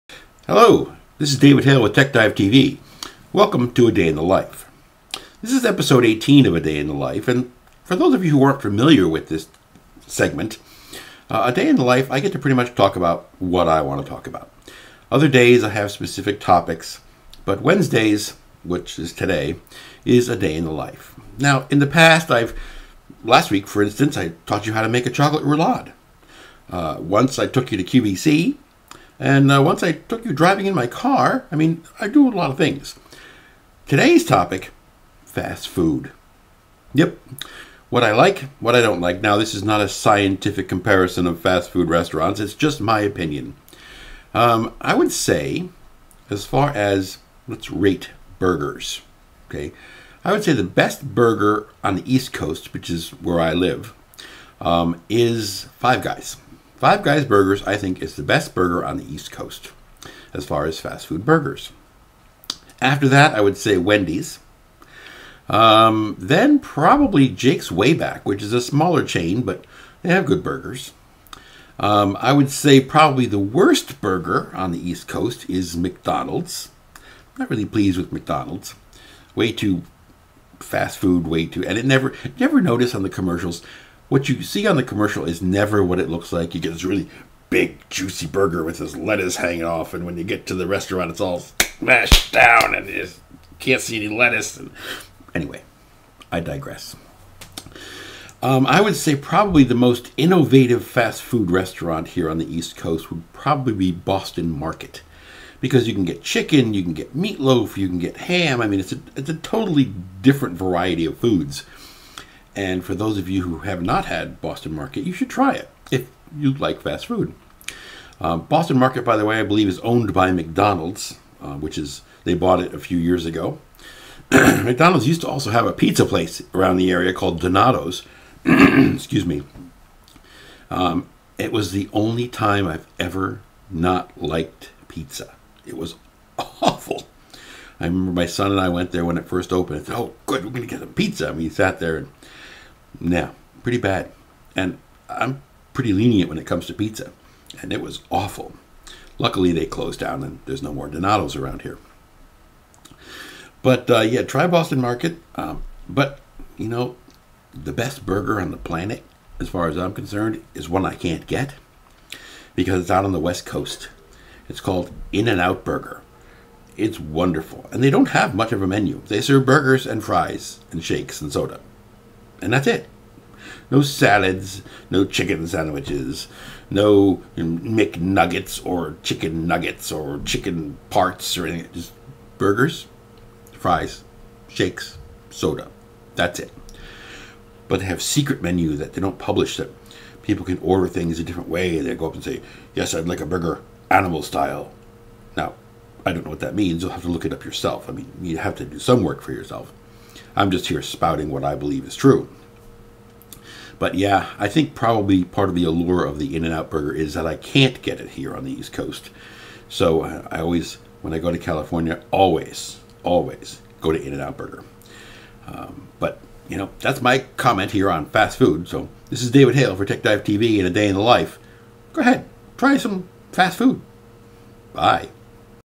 A Day in the Life Episode 18
This episode of TechDiveTV "A Day in the Life" talks about an American obsession; Fast Food!